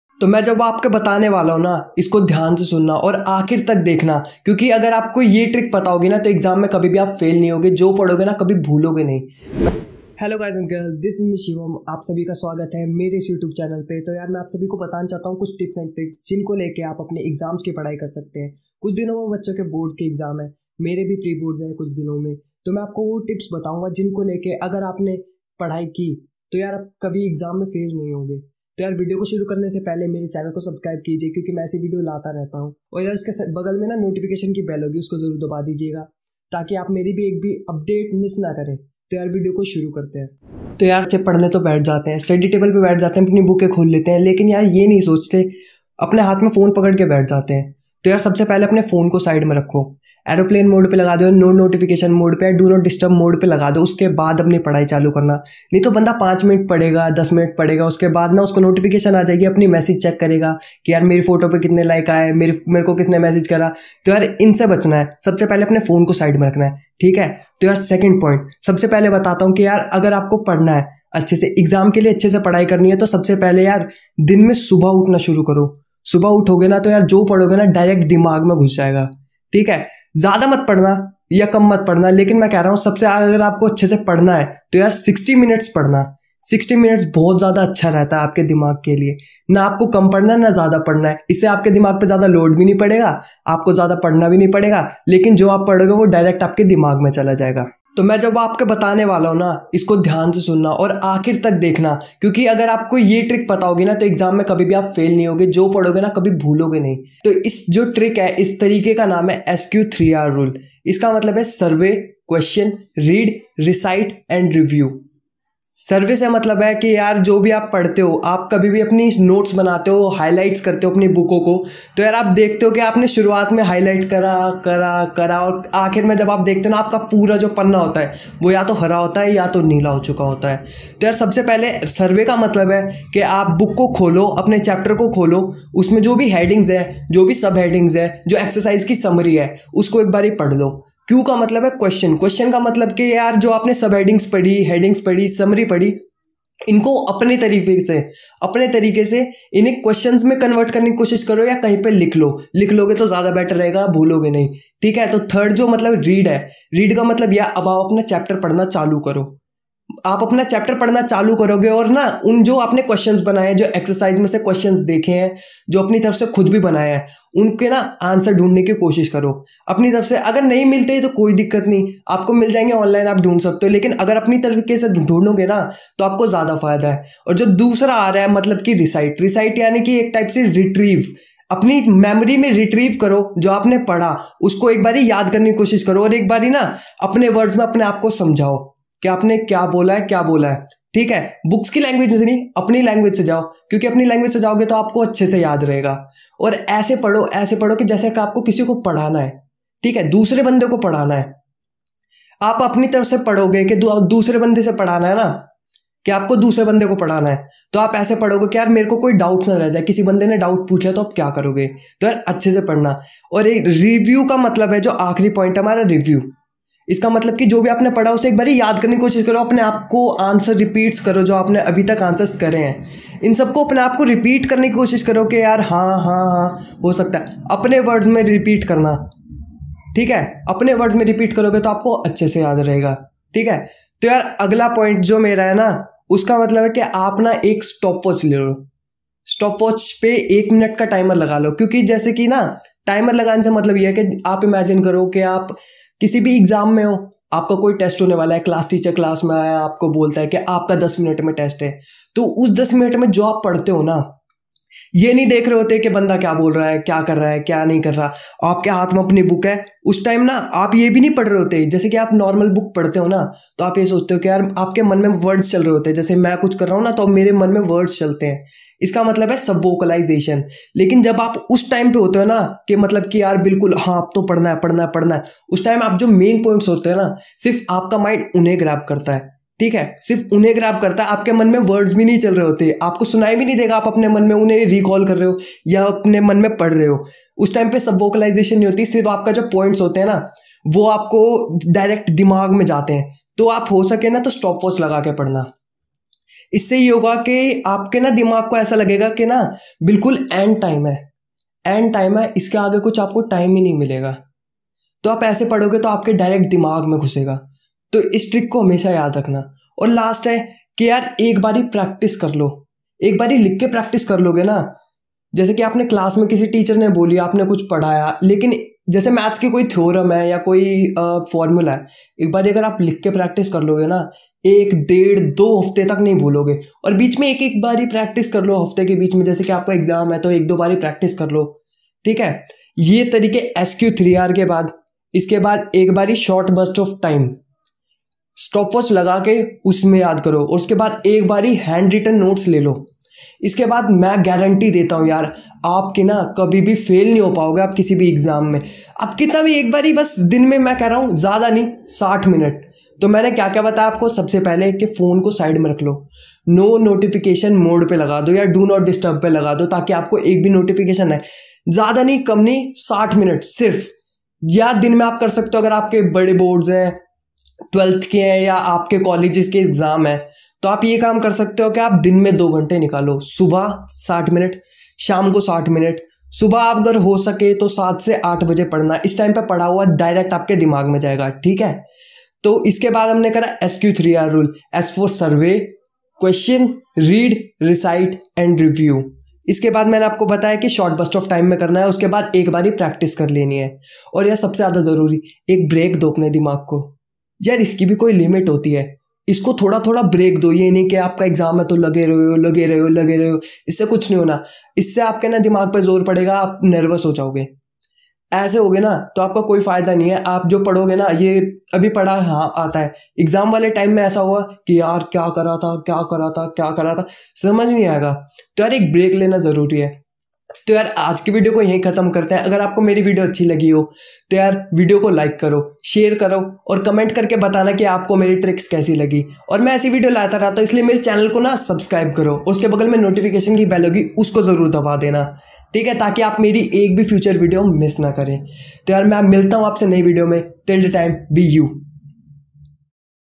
Boost your learning speed || Tips for students watch till end || Tips for exams [] IAmShivamXSood
Today I will tell you how to focus on studies Boost your learning speed drastically SQRRR rule will help you taking notes and studying for exams.